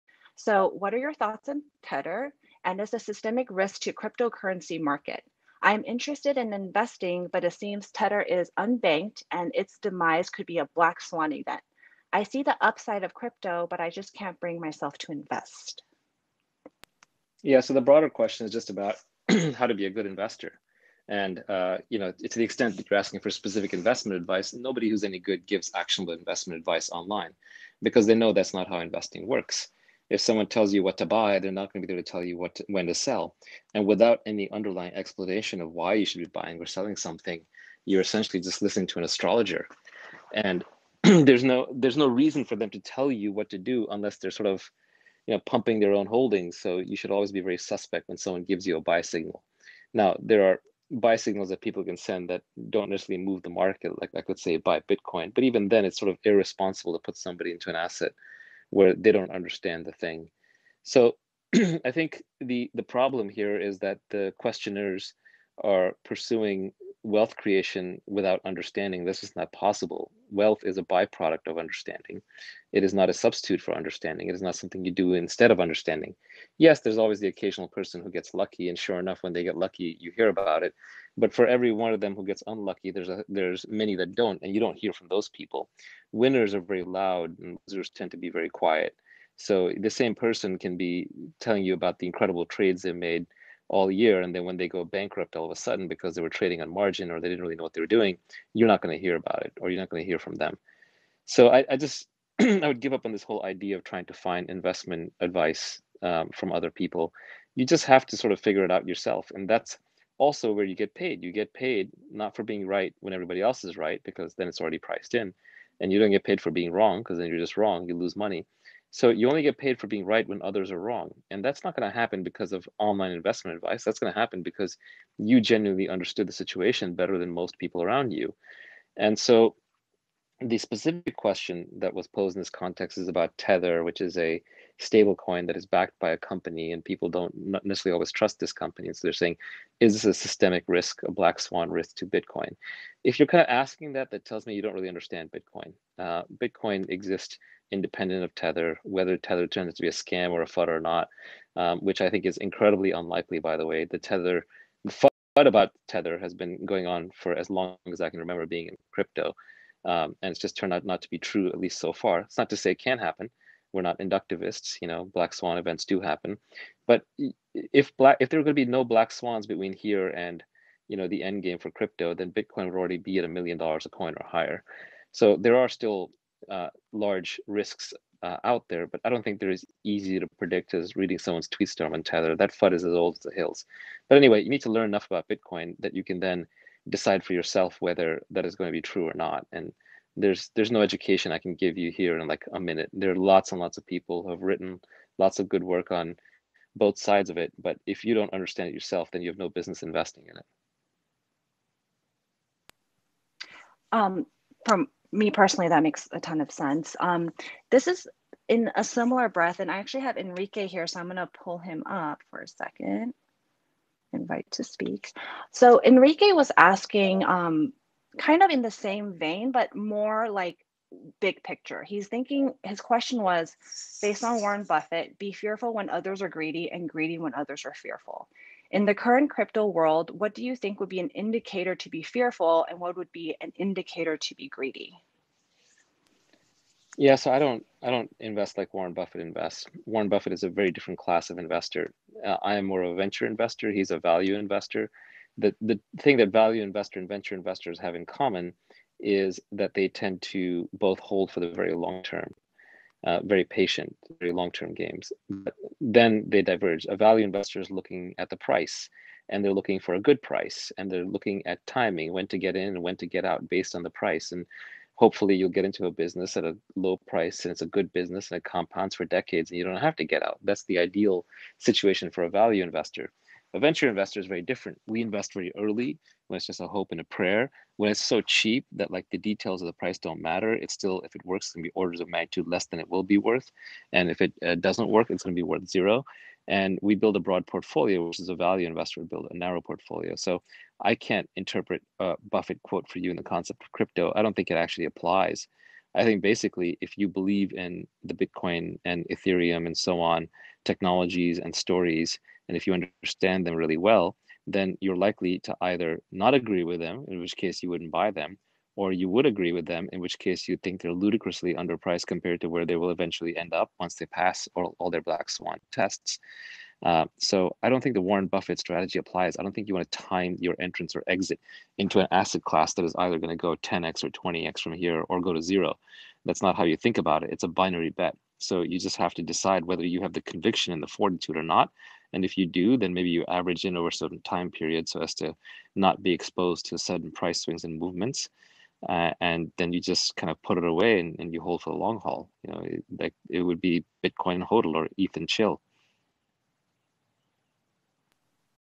Naval Ravikant gives crypto currency investment advice
Clip from Clubhouse AMA March 31. 2021